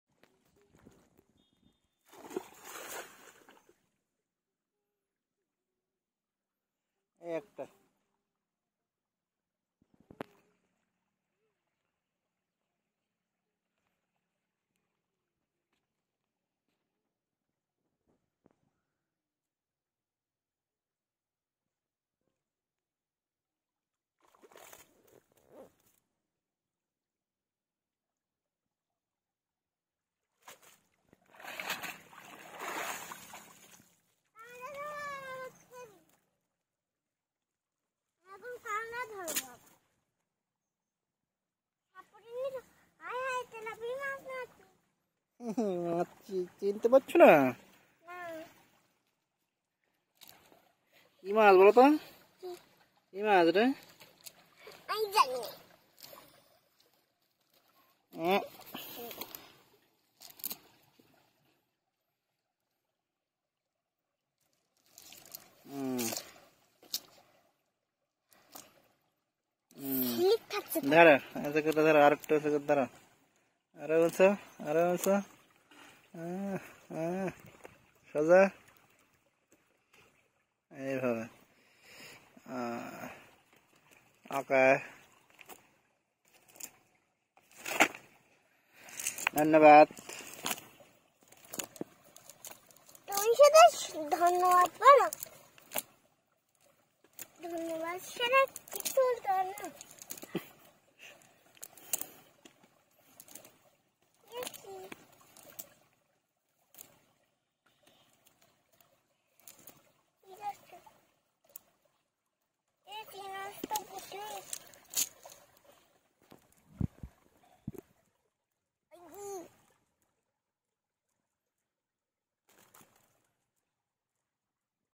Fishing by Net in fishery
Village boy is catching fish by net in outside fishery. He is getting Tilapia, prawn and parshe fish.